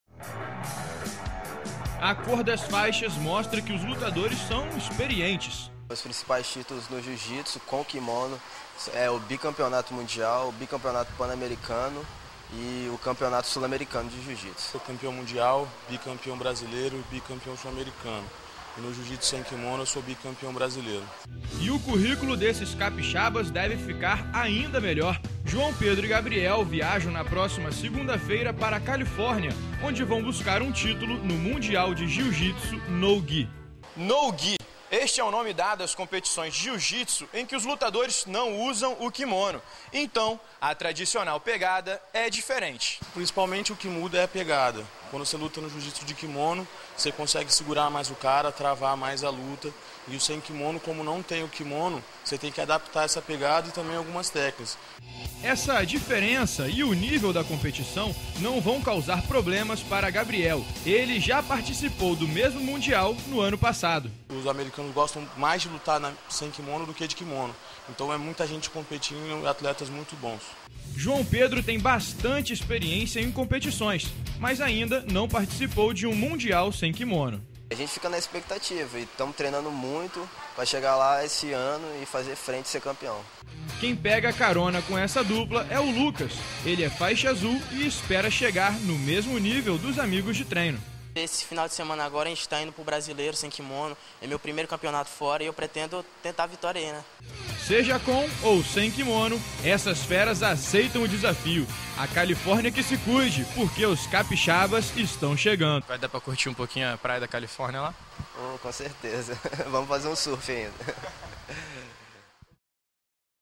Do Espírito Santo para a Califórnia
João Pedro "Somália" e Gabriel Lucas vão encarar o mundial de jiu jitsu no  gi (sem quimono) nos EUA. A modalidade é torna a luta um pouco mais difícil, já que a pegada é diferente. Mas os dois capixabas estão preparados.